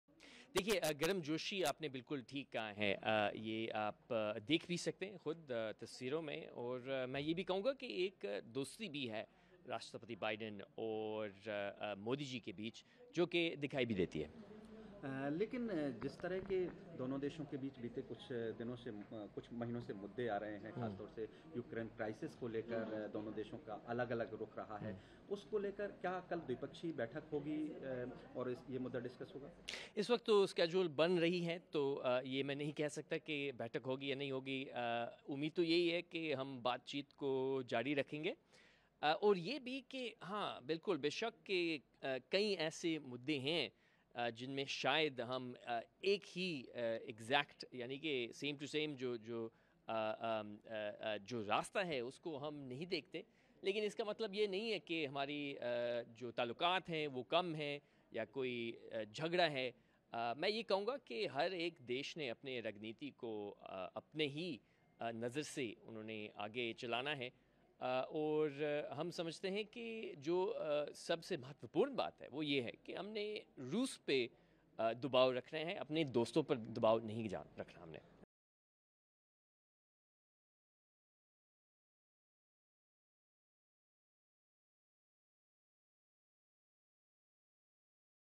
“On every issue we are together…” US State Department Spokesman Zed Tarar on Modi-Biden meet
US State Department Spokesperson Zed Tarar on November 15 said that India and United States have always been on the same pages’ as far as global issues are concerned. “There is a friendship between President Biden and PM Modi which is apparent. There are several such world topics where both nations do not see face to face. But that does not affect our relationship. Every country moves as per their own strategy, most imp is we are keeping pressure on Russia, not on our friends,” said Zed Tarar.